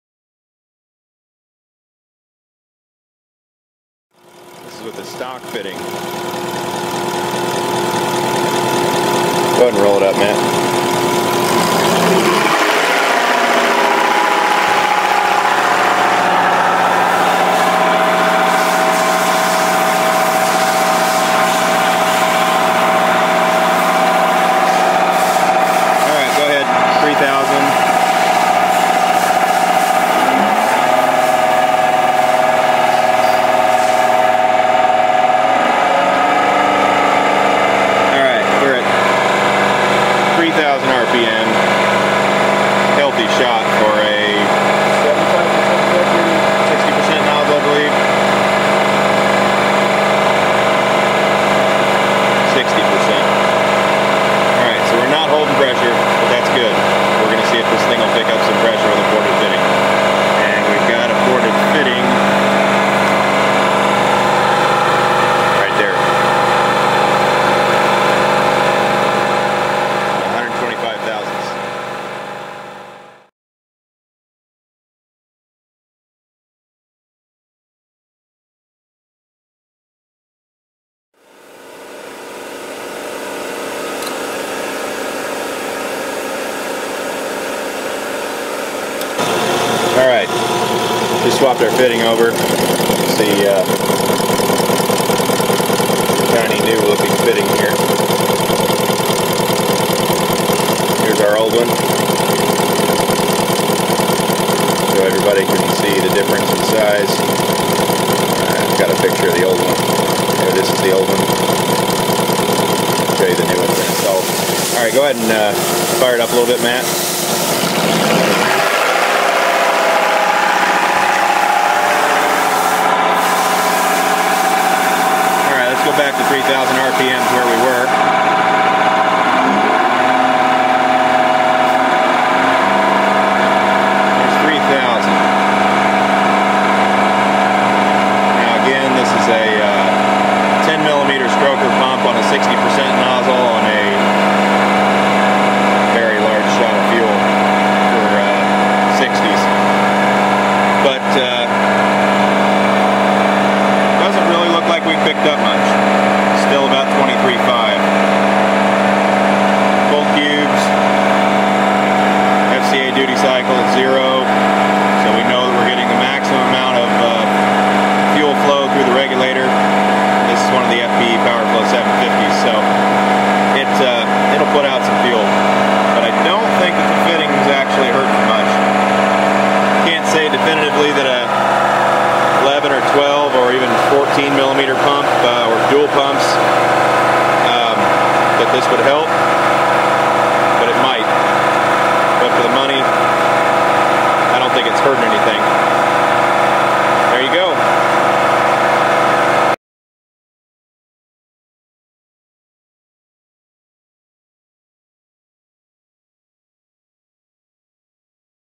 Ported Fuel Rail Fitting Test Fast Motion
Fleece Performance Engine Simulator / Test Stand being put to use to test the popular modification of fuel rail inlet fittings.